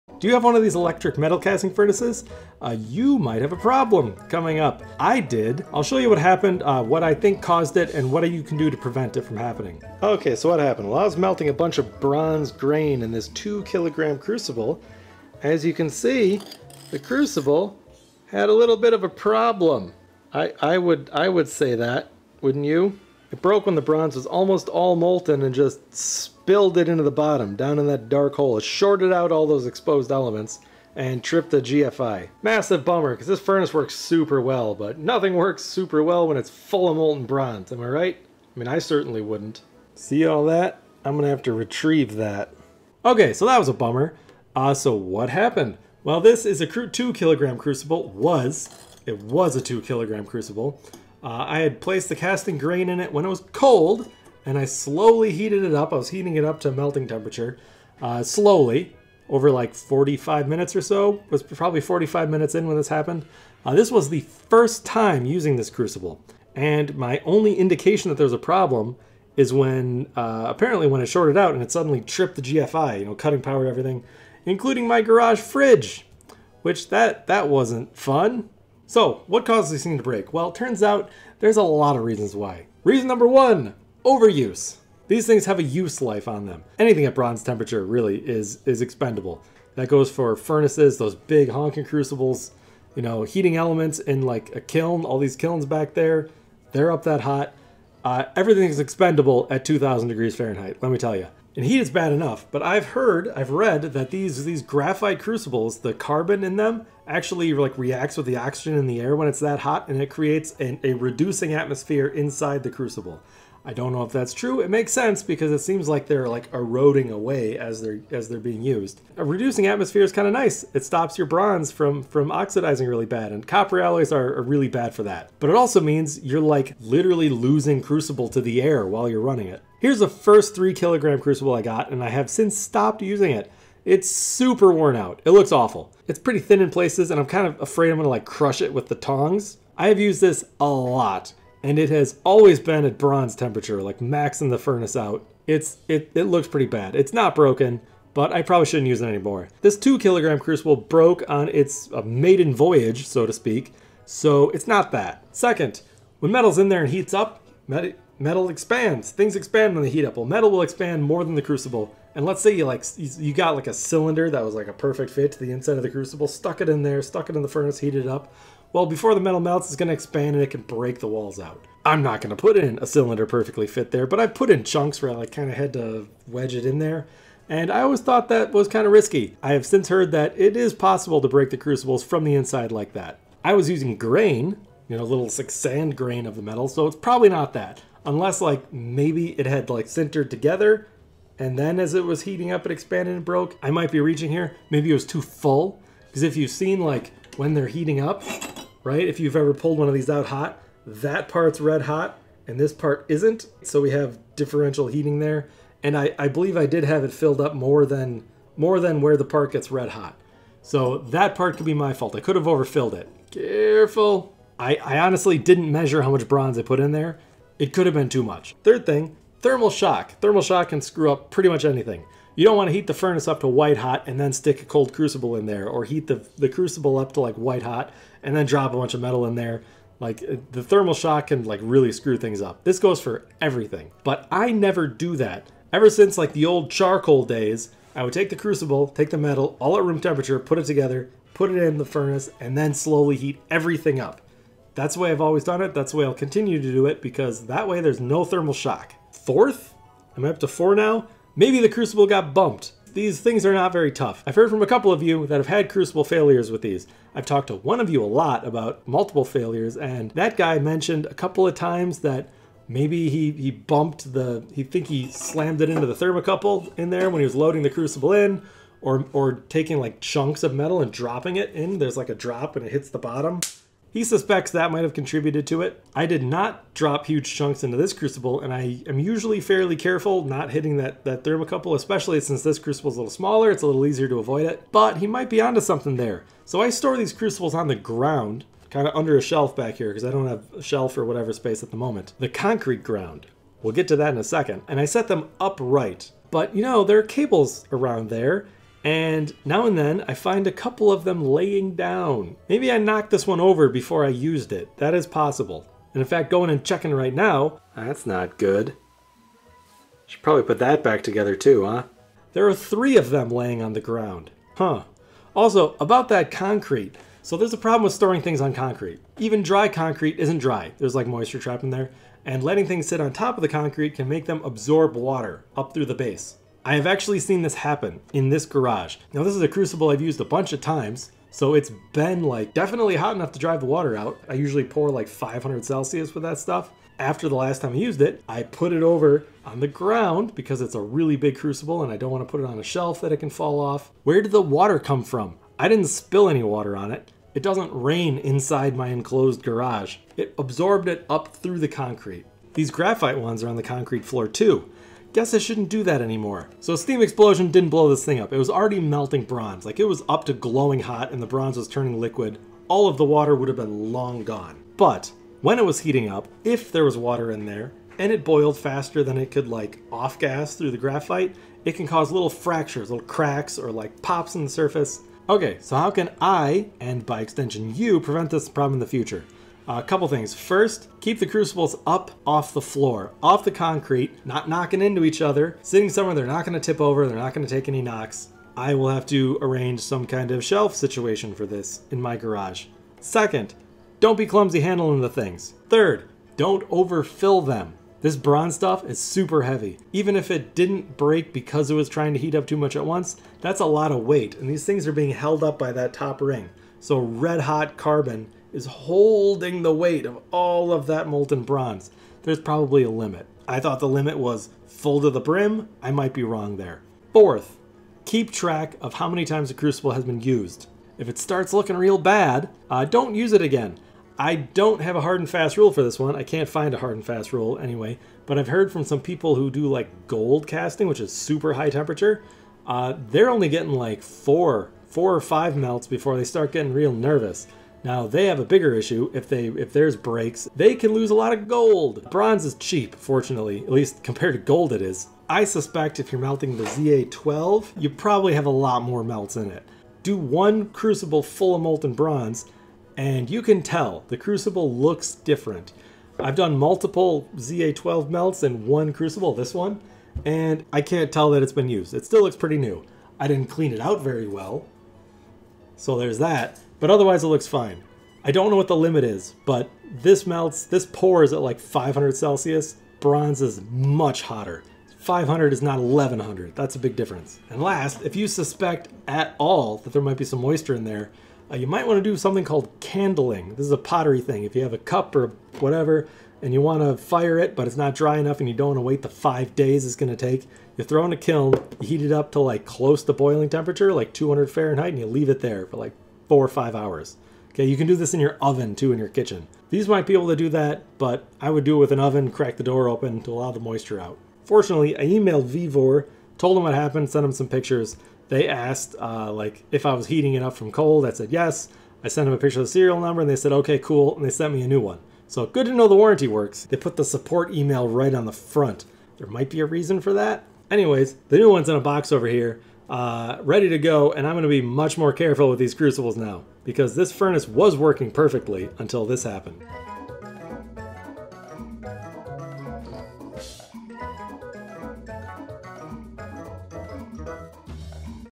The Problem With Electric Metal Casting Furnaces
Spills are messy.  Especially Bronze Spills...

Want to learn sand casting using your 3D printer? I can teach you!